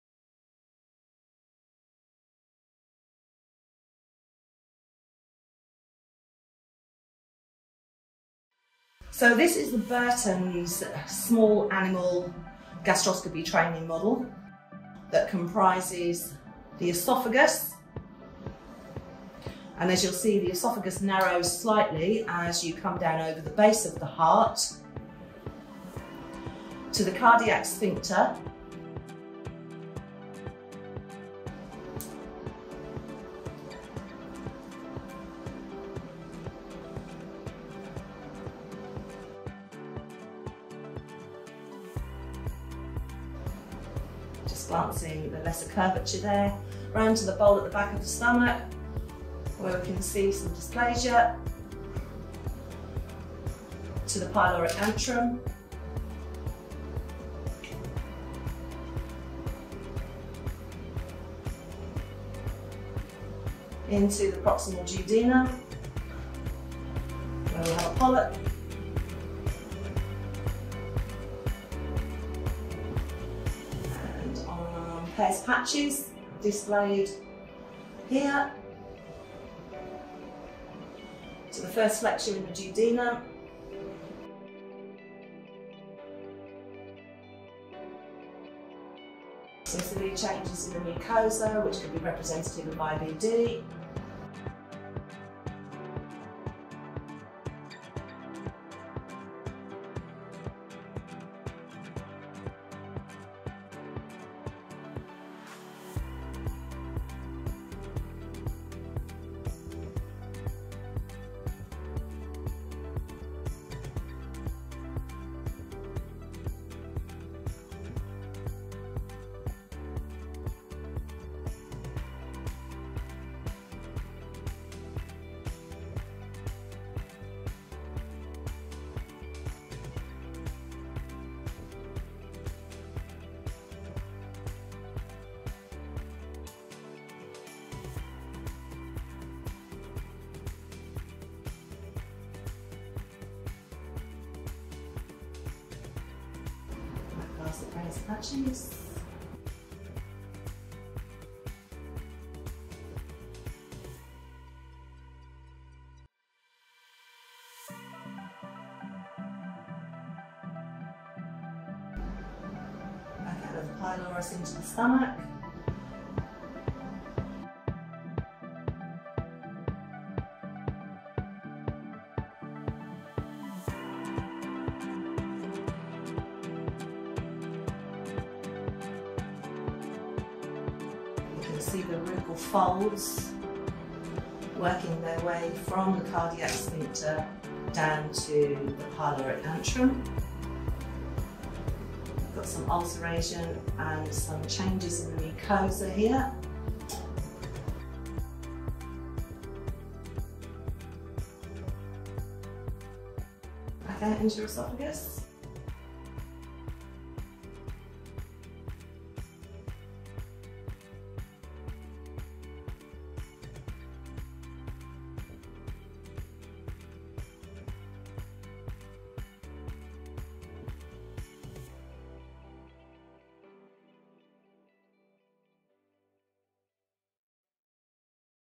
Take A Tour Through Our Small Animal Gastroscopy Training Model
The Burtons Small Animal Gastroscopy Training Model is based on a 25-kilo dog and contains a range of clinical disease states and pathology for veterinarians to investigate.